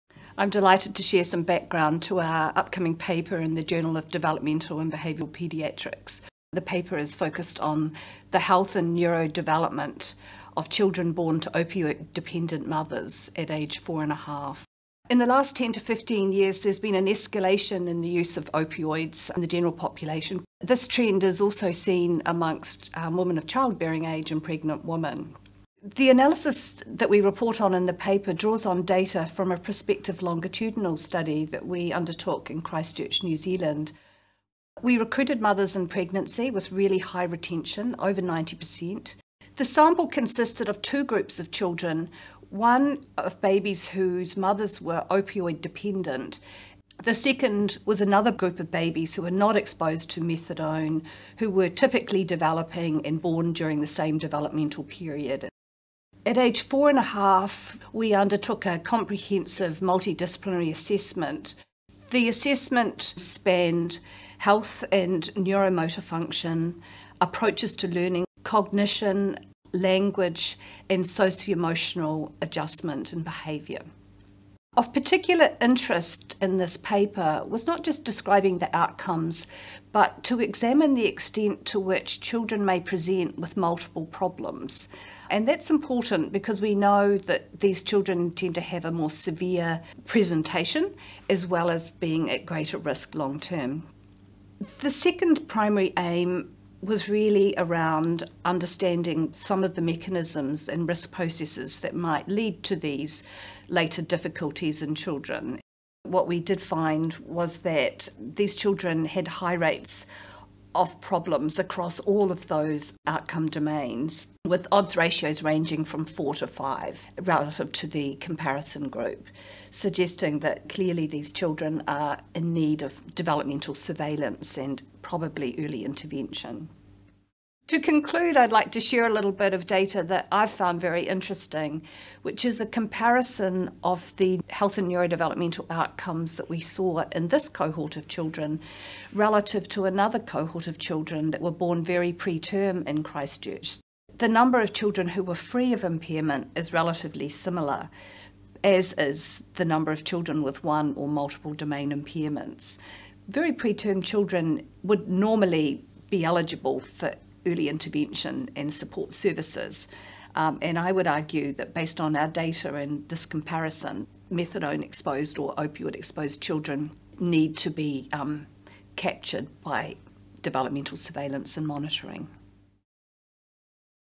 Health and Neurodevelopment of Children Born to Opioid Dependent Mothers at School Entry
Children born to opioid-dependent mothers are at high risk for poor growth and neonatal withdrawal, but little is known about their longer term outcomes. We examined the health and neurodevelopmental outcomes of a cohort of 100 children born to opioid dependent mothers during pregnancy. These mothers and children were followed prospectively from pregnancy to age 4.5 years alongside a comparison group of 110 randomly identified non-methadone-exposed children and their mothers. Findings highlight the complex developmental needs of these children. The risk of later problems was higher for male children born to higher social risk mothers who used more licit and illicit substances during pregnancy and when children were raised in lower quality home environments. Read the article